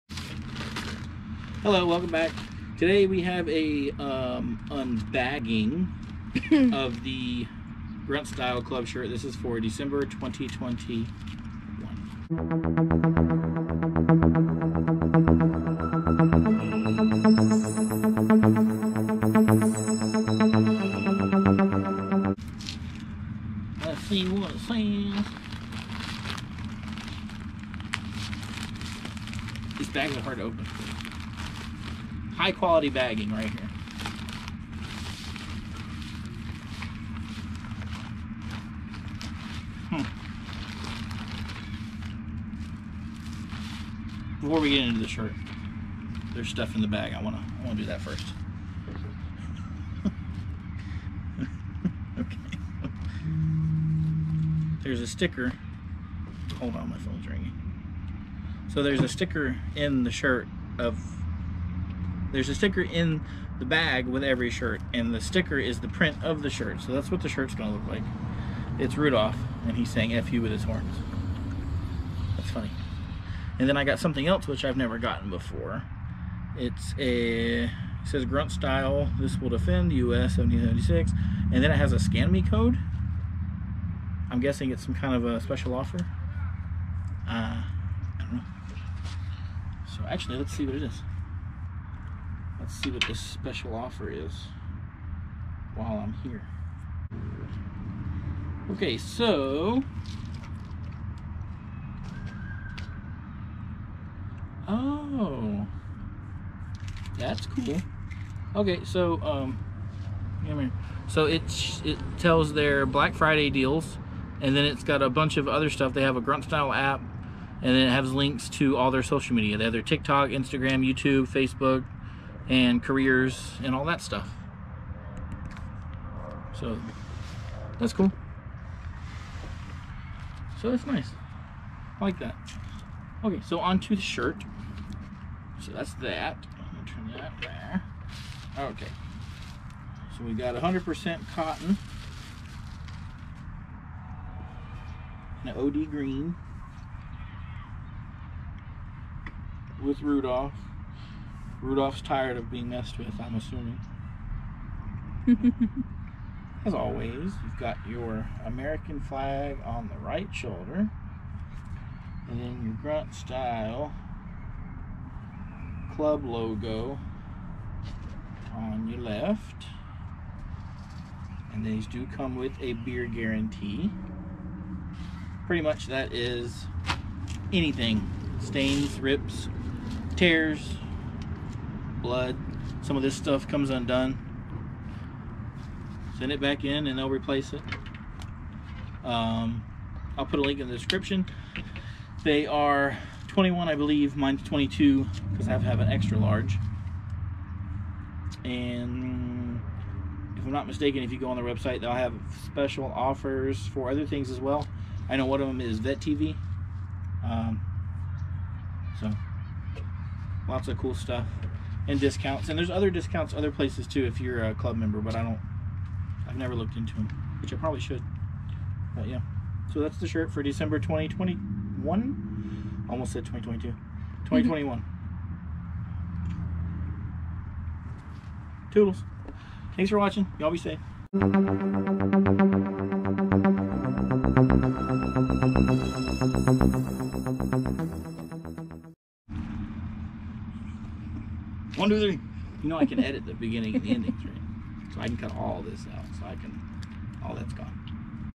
Club Grunt Style Shirt December 2021
Hey y'all sorry for the late posting. I forgot to post it. My bad. Anyway here is my Dec 2021 Club shirt video. Hope you enjoy. This one has a bit of humor in it so I hope you like it.